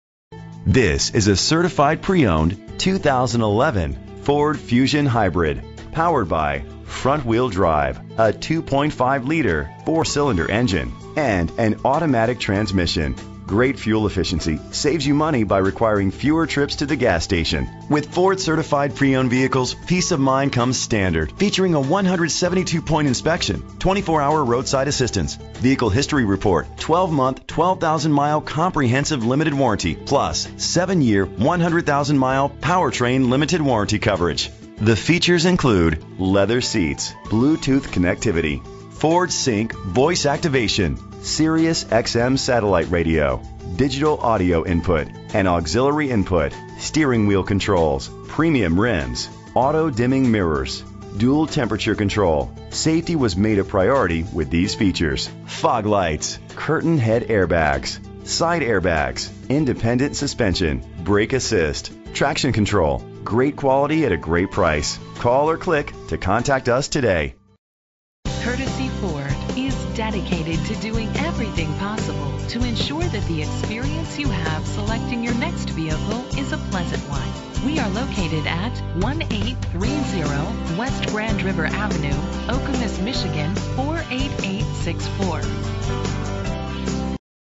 2011 Ford Fusion Hybrid - Okemos MI
Phone:
Year: 2011
Make: Ford
Model: Fusion Hybrid
Trim:
Engine: 2.5 liter inline 4 cylinder 16 valve
Transmission: Automatic CVT
Color: Sterling Gray Metallic
Mileage: 50999
Address:
1830 West Grand River Ave.